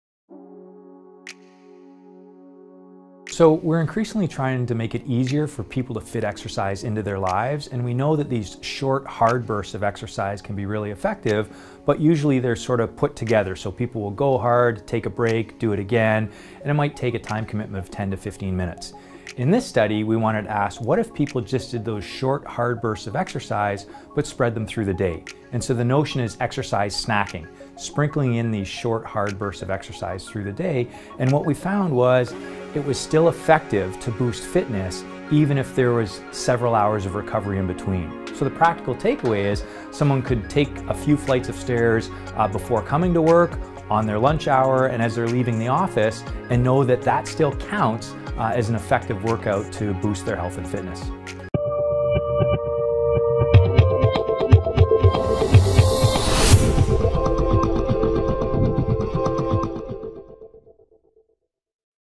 Exercise Snacks
McMaster's Martin Gibala talks about short simple breaks of exercise.